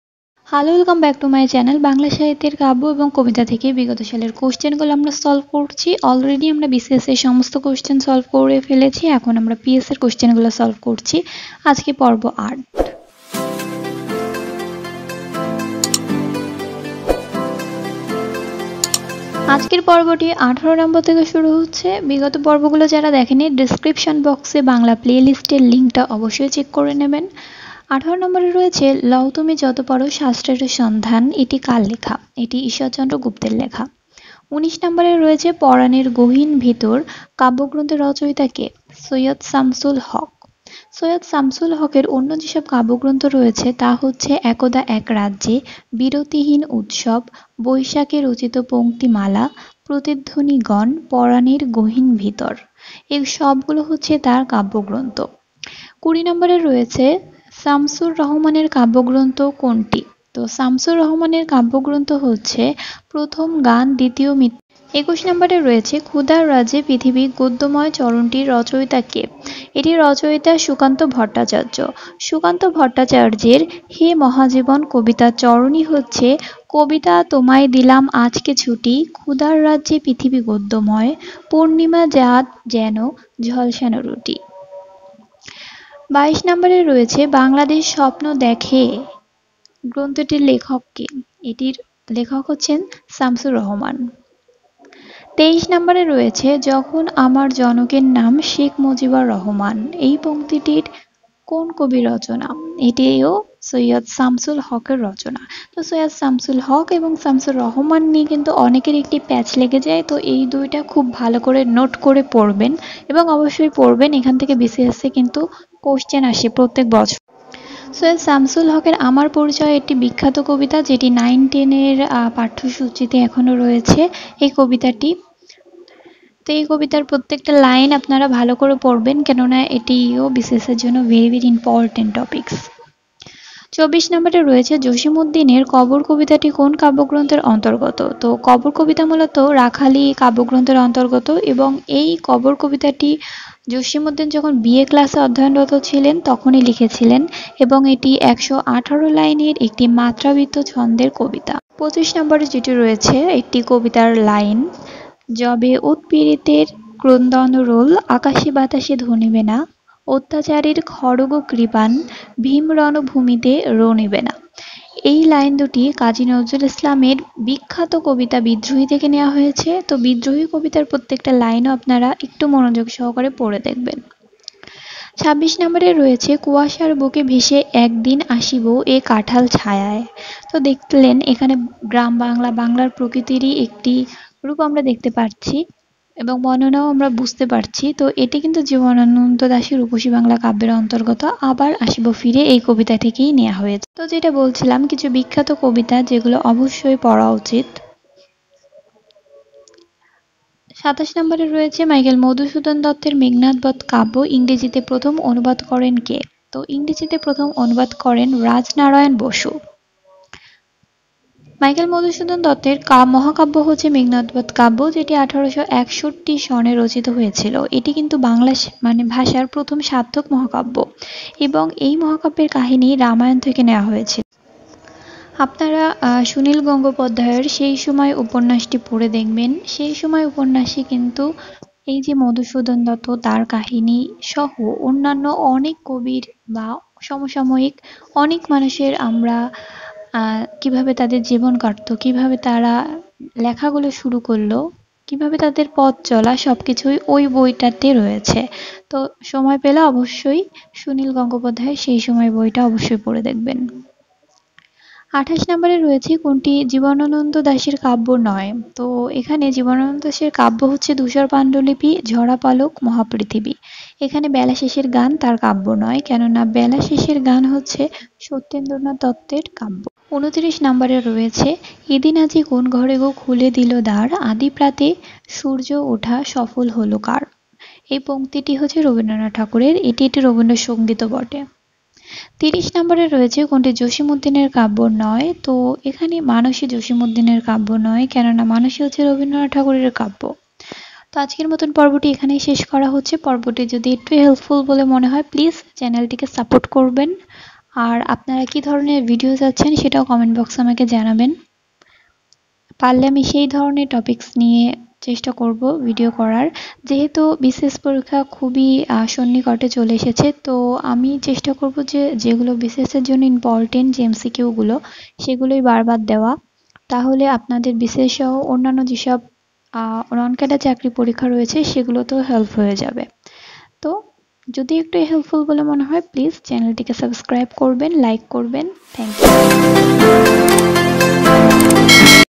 বাংলা কাব্য ও কবিতা থেকে বিগত সালের প্রশ্ন সমাধান | পর্ব-০৮
বিসিএস সহ অনেক পরীক্ষাতে বাংলা সাহিত্য খুব গুরুত্বপূর্ণ।
প্র‍্যাক্টিস করার জন্য ভিডিওটি ফলো করতে পারেন।
যাদের বই নেই বা পড়তে ইচ্ছা করেনা,তারা এই ভিডিও এর মাধ্যমে শেষ করে ফেলতে পারলেও অনেকটা এগিয়ে থাকা যাবে।
আজকের আলোচনা থেকে কোন প্রশ্ন থাকলে কমেন্টে জানাতে পারেন।

Be part with My YouTube channel.